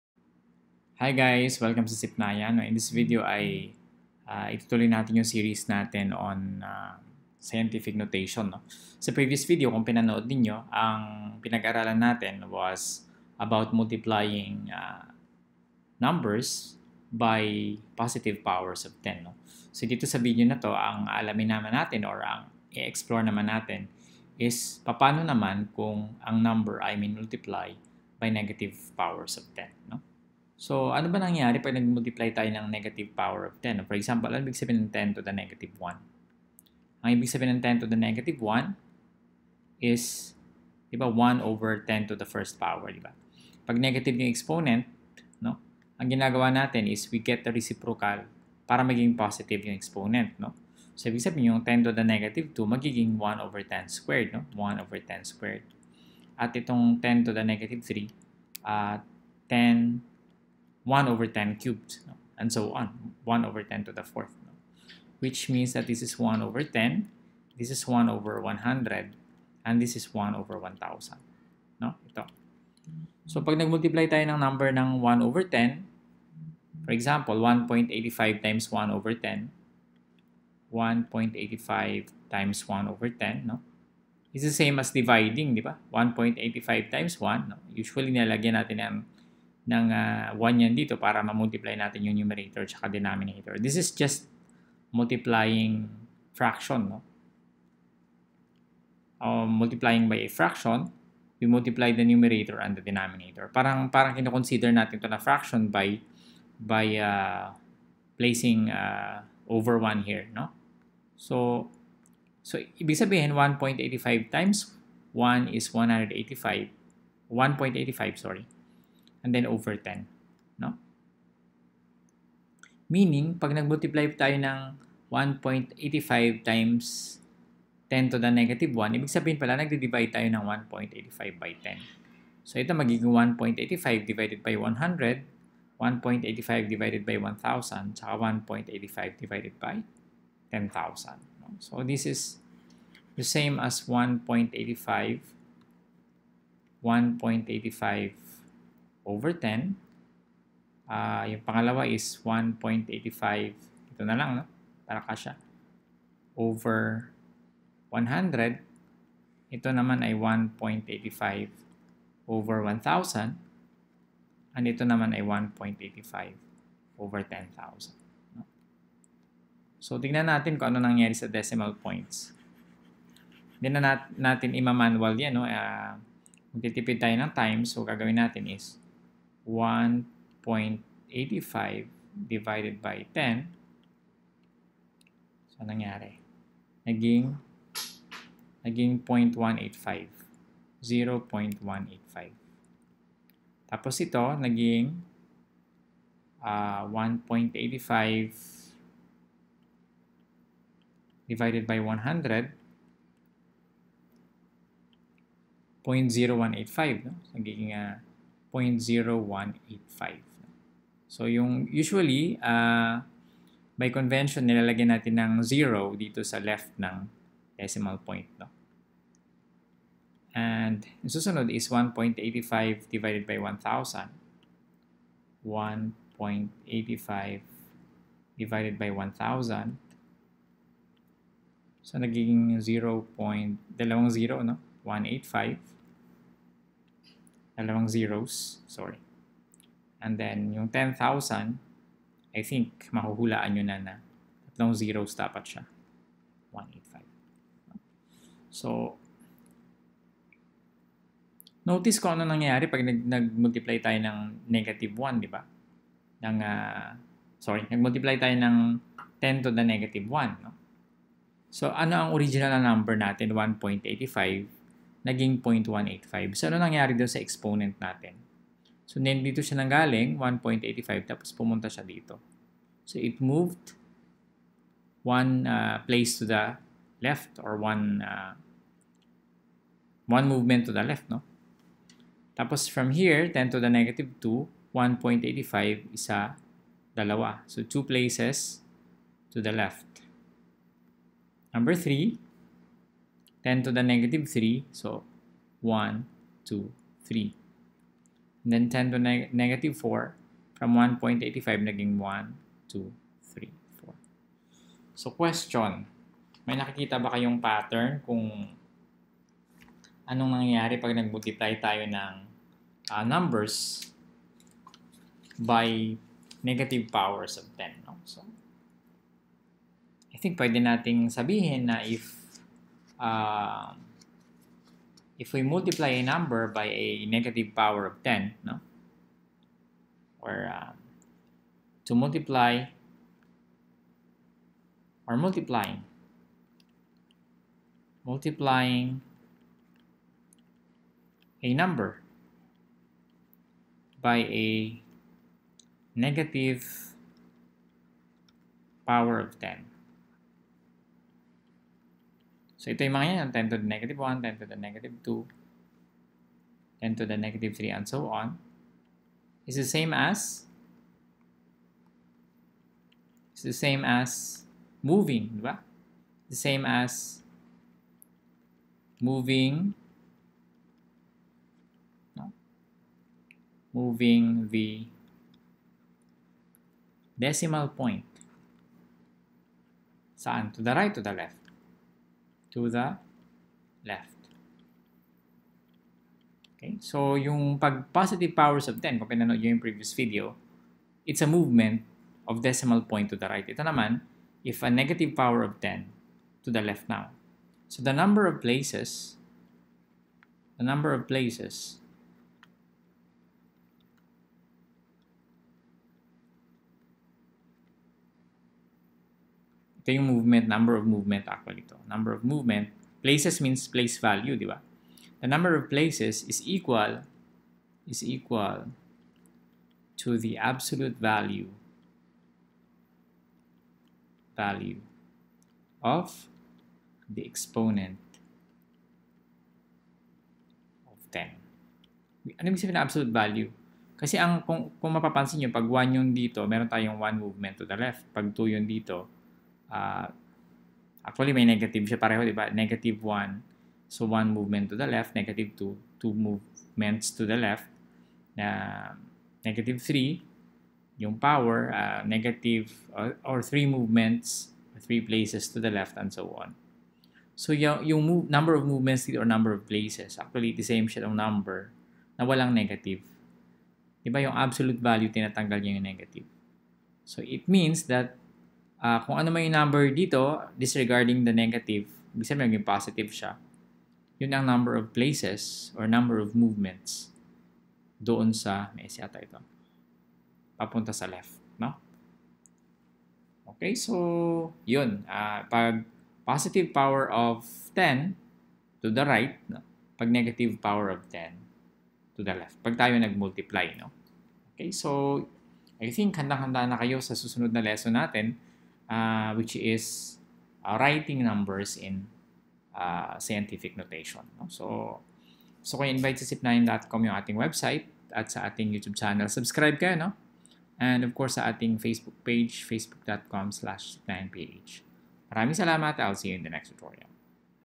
SCN03 Multiplying Numbers by Negative Powers of 10
In this video, we are going to investigate the effects of multiplying a number by negative powers of 10.